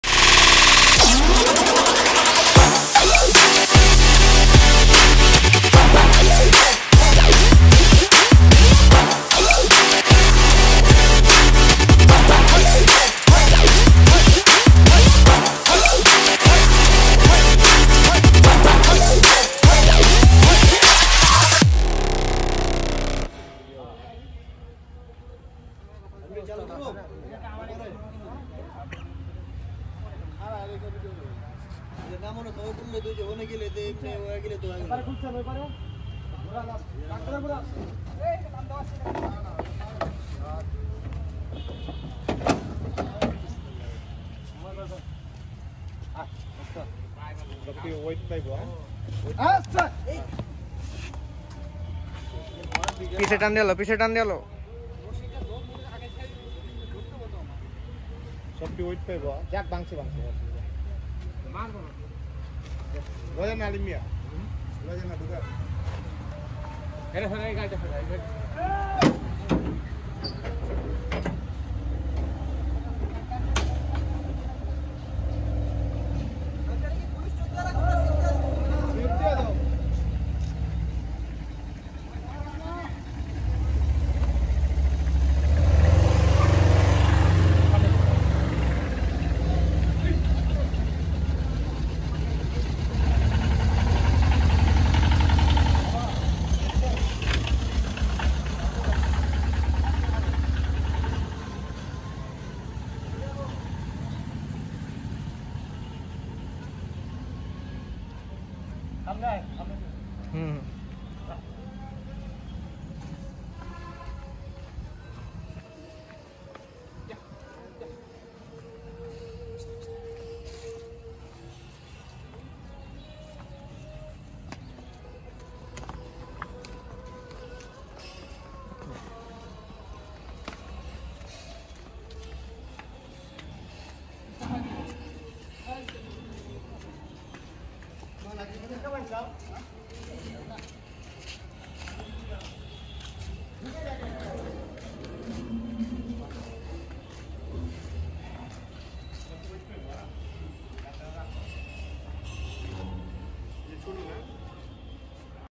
Beautiful Black Sahiwal | Unloading Time | New Arrival of Apple Agro | Qurbani Collection 2021
Dhaka Cow Mania presents the unloading video of the beautiful black Sahiwal which just arrived at Apple Agro for 2021 Qurbani Eid